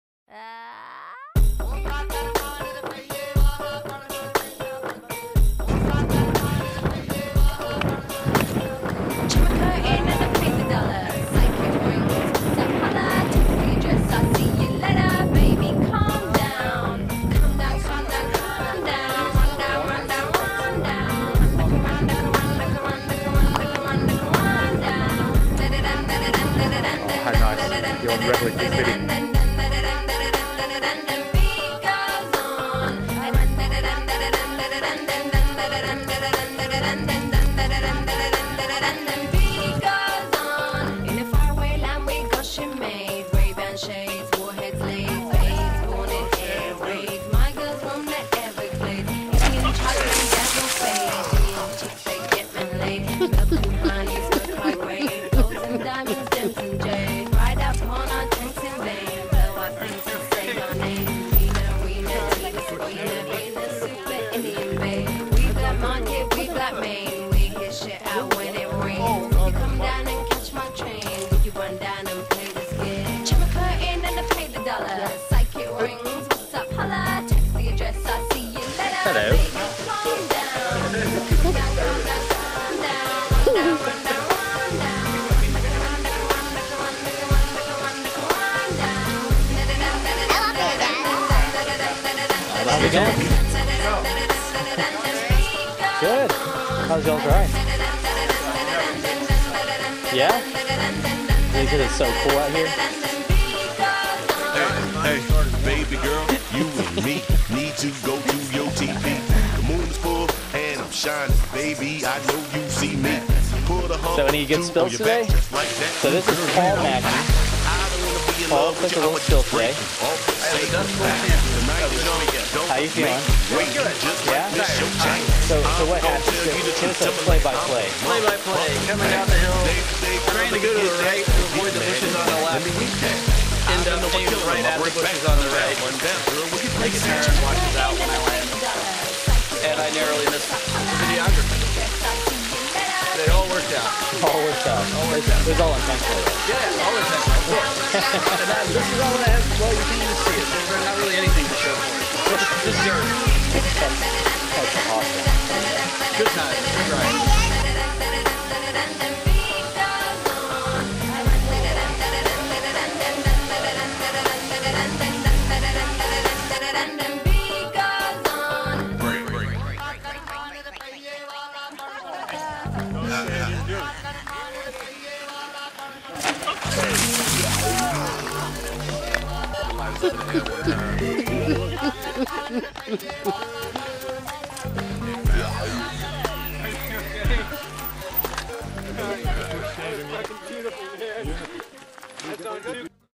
A Day of Riding at Reveille Peak Ranch by Mojo 7-23
A view from the trails and the pavilion at Reveille Peak Ranch, near Burnet, Texas, with 100K of trails (including 15 miles of hand-cut single track), including a super D and a soon-to-be-complete .6-mile true downhill and skills park. Riders of all skill levels will find a fun challenge!